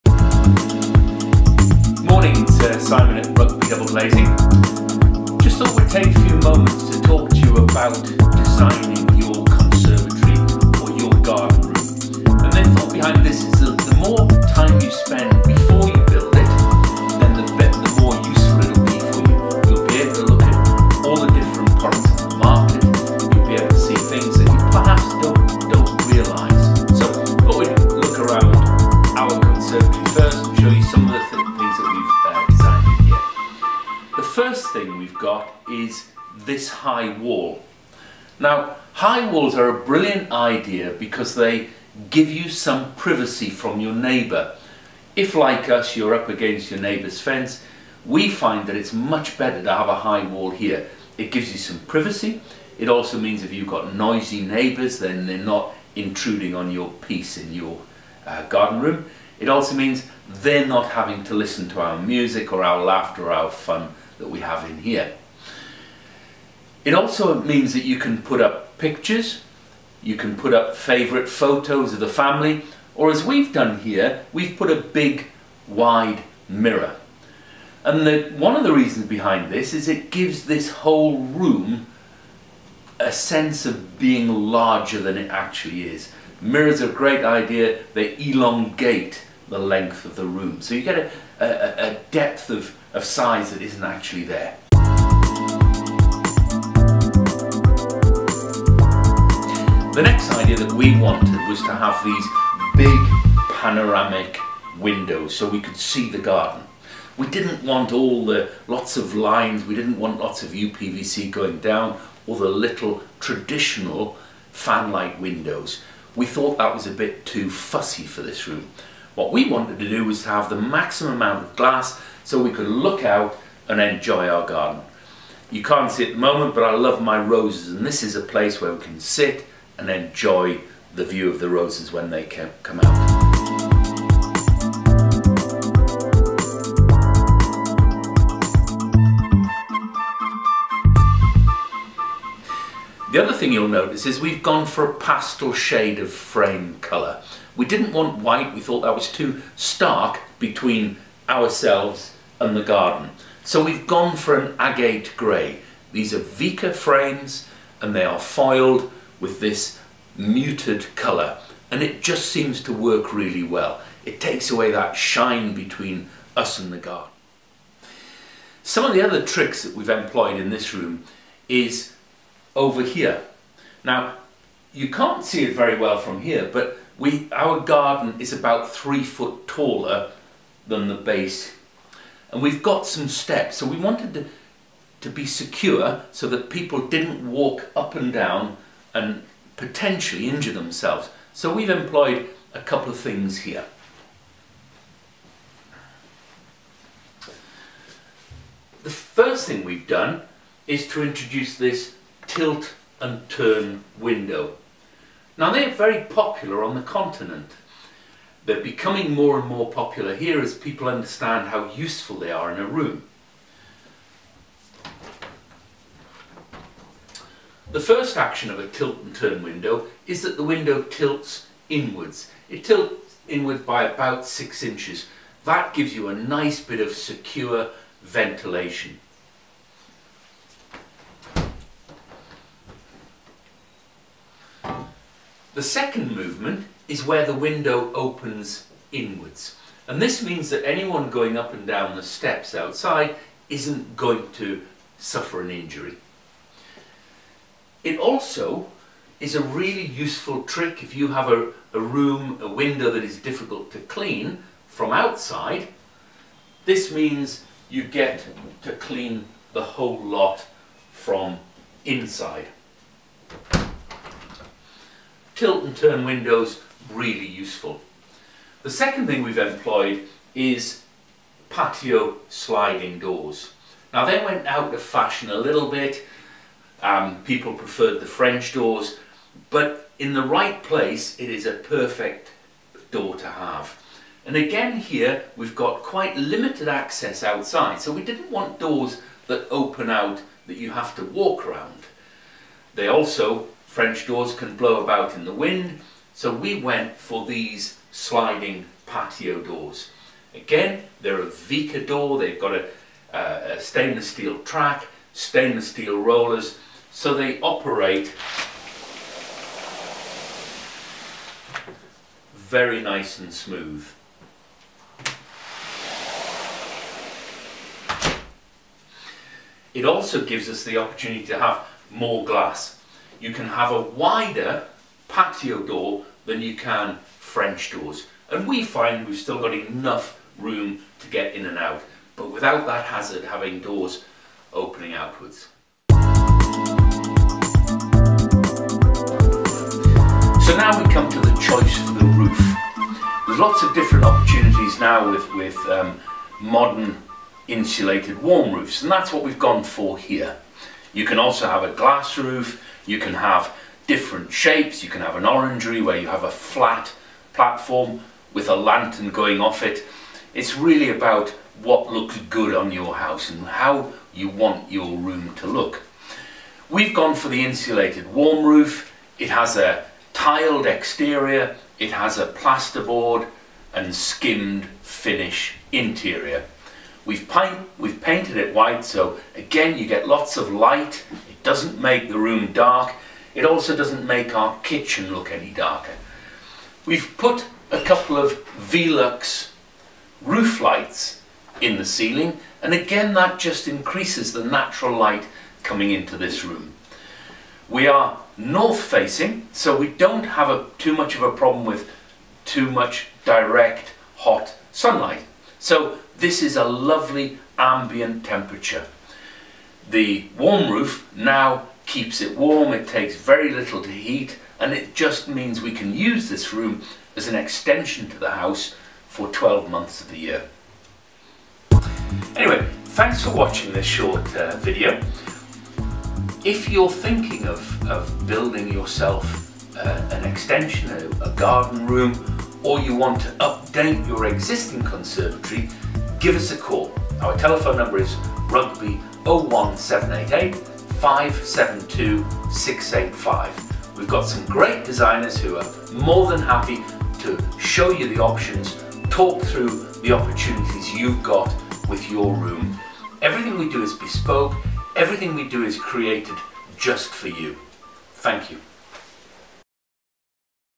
Rugby Double Glazing - Garden room extension tour
Welcome to Rugby Double Glazing, Our Director Simon gives you a tour of a garden room extension built by our wonderful team.